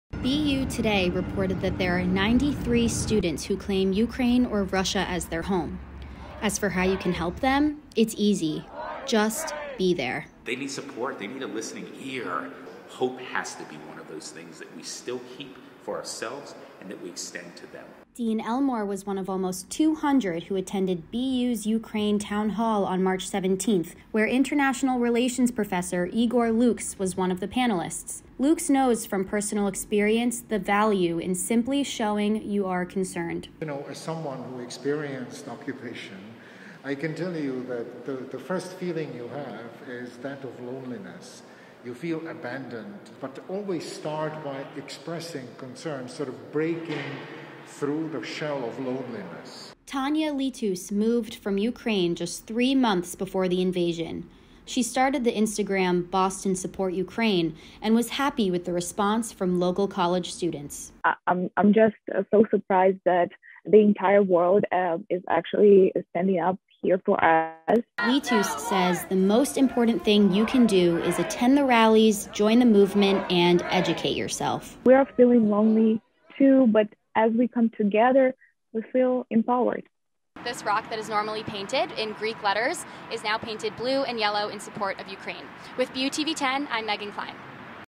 Boston University's community helps students impacted by Russia's invasion into Ukraine
Russia invaded Ukraine on February 24 and since then, the Boston University community has been looking for ways to help their impacted students. BUTV10's Megan Klein has the latest.